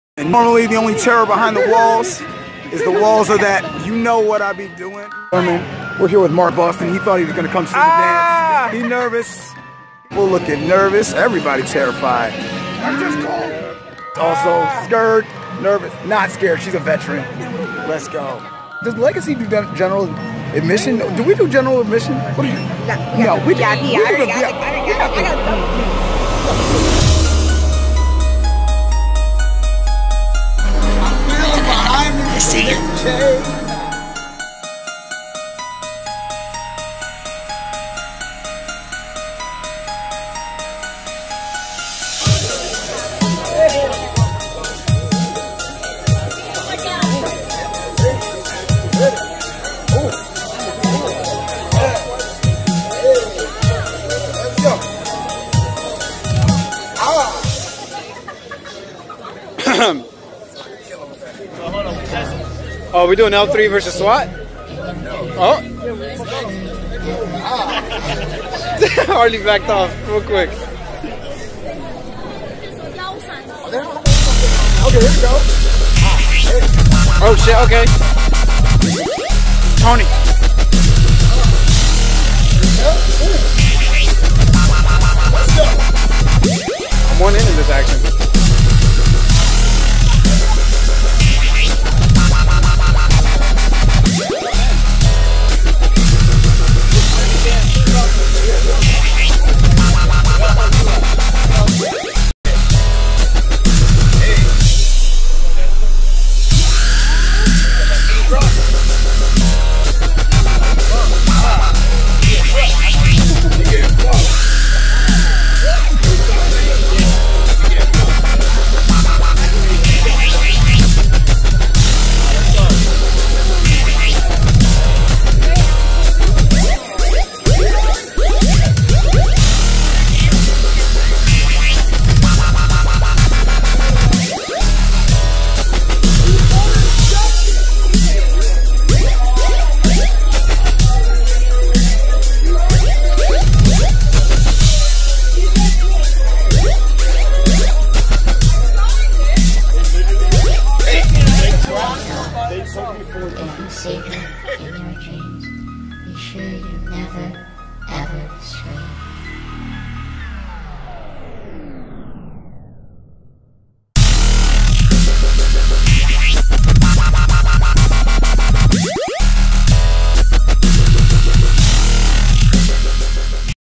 L3 vs. Zombie S.W.A.T. Dance Battle (Eastern State Penitentiary) @inferno1431 /
Just My @l3gacydance Crew & I enjoying some spookyness the only way we know how!  DANCE! @deivindancesalot & @inferno1431 putting in work!

Filmed by: @tonipalma11
Edited by: @inferno1431 @l3gacydance

Also Featured in the video:
@smartmark - Mark Boston
@tonipalma11 - Toni Palma
@lj324 - Lisa Jones
@swiftarrow23 - Jonathen Bacca
@fit-Faery - Lauren Stefan
@malachi1998 - Malachi Oliver
@daniellecarty - Danielle Carty